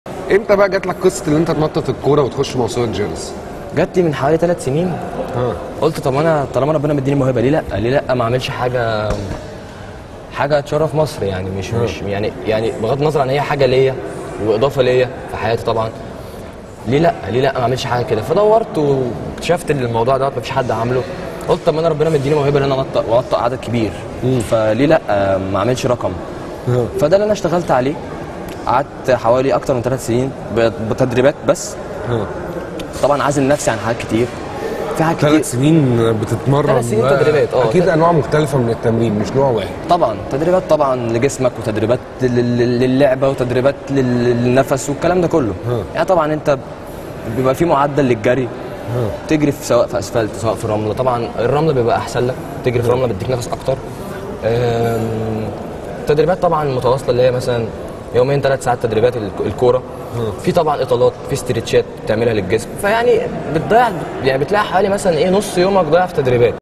سبوت اون سبورت | سانتوس وبداية فكرة موسوعة جينيس
عبد المنعم ( سانتوس )
بطل العالم في التحكم بالكرة بموسوعة جينيس

Spot On SPORT - سبوت اون اسبورت